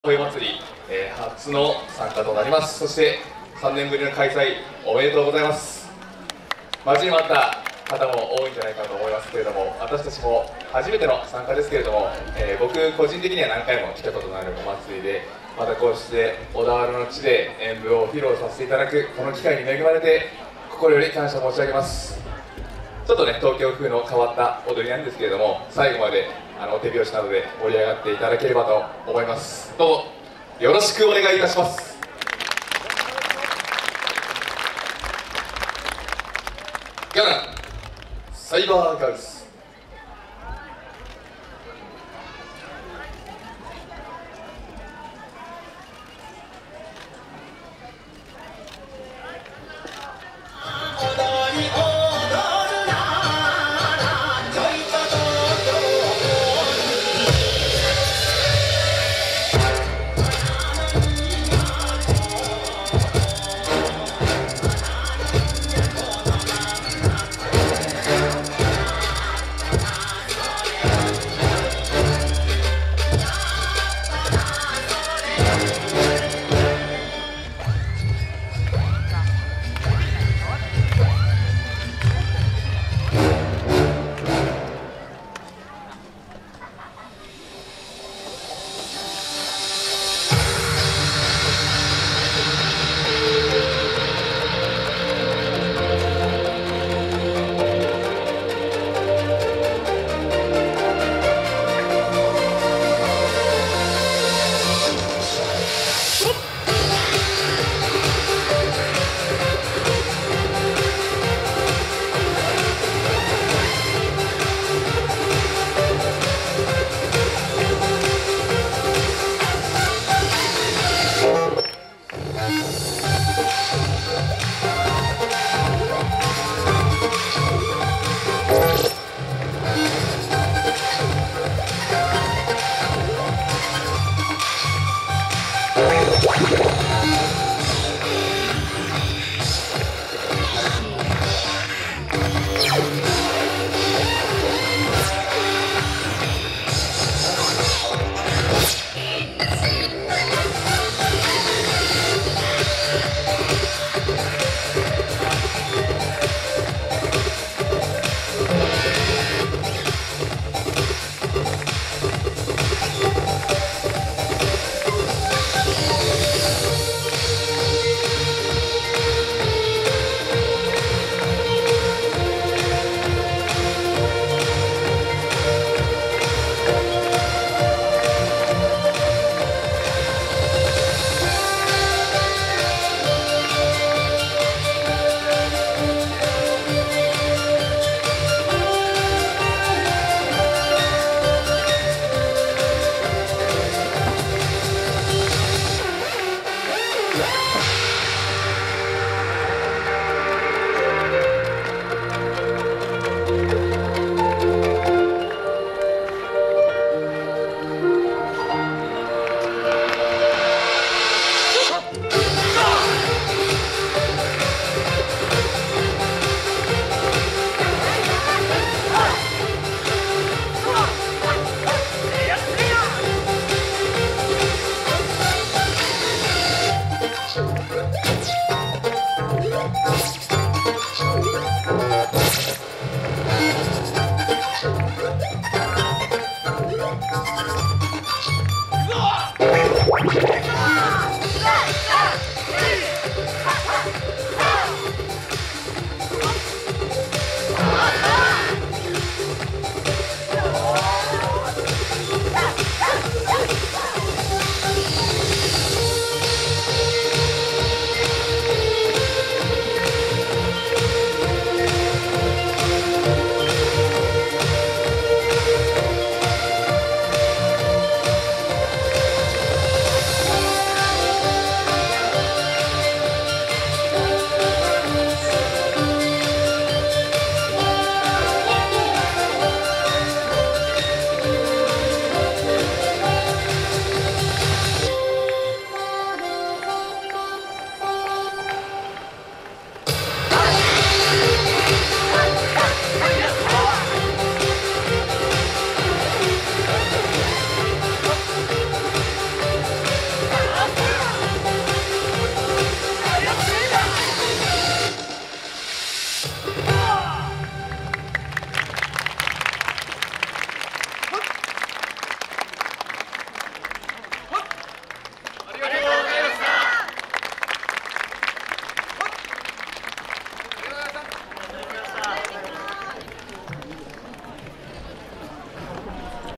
GARAN43/35°（小田原お堀端万葉の湯賞）【APSC広角_R_4K】／三の丸ホール広場・コンテスト演舞／小田原えっさホイおどり2022
2022/11/12（令和4年11月12日）
ODAWARAえっさホイおどり2022
会場：小田原城三の丸ホール広場コンテスト会場
主催：ODAWARAえっさホイおどり実行委員会
共催：小田原箱根商工会議所 青年部
後援：小田原市、小田原箱根商工会議所、（一社）小田原市観光協会、小田原市教育委員会